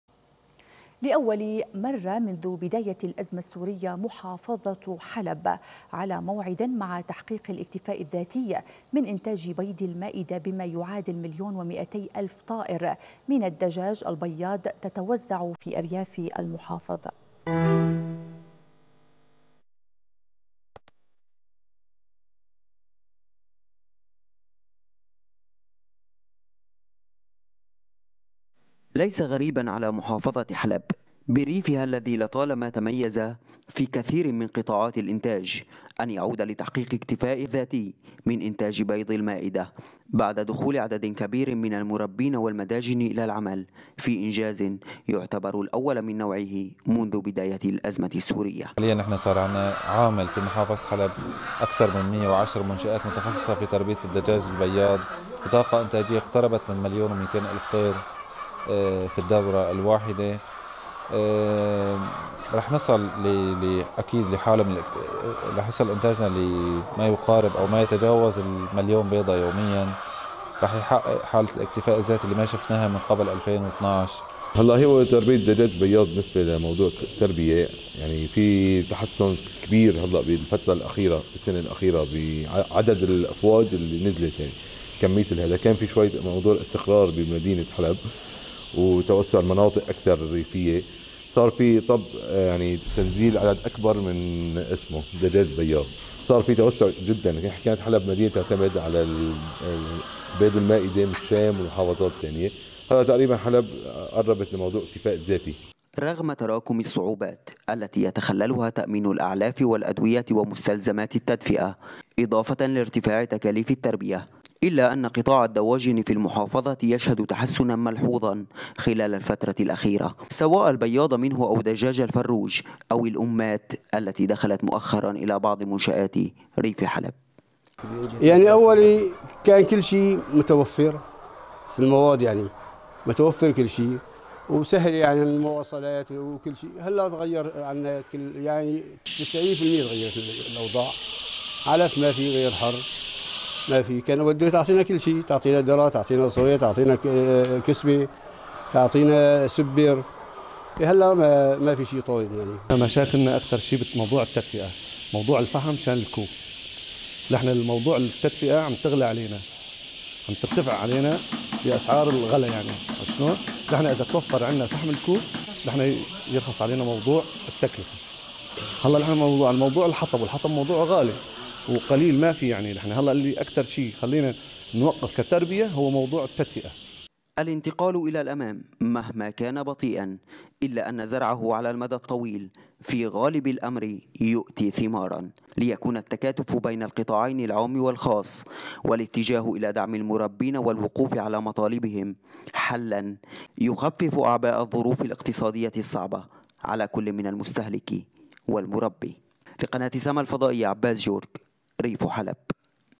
لأول مرة منذ 12 عاما محافظة حلب وريفها على أعتاب الاكتفاء الذاتي من إنتاج بيض المائدة
لأول مرة منذ  12 عاما
محافظة حلب وريفها على أعتاب الاكتفاء الذاتي من إنتاج بيض المائدة

تقرير : عباس جورك

محمد ياسين - رئيس دائرة الإنتاج الحيواني في مديرية زراعة حلب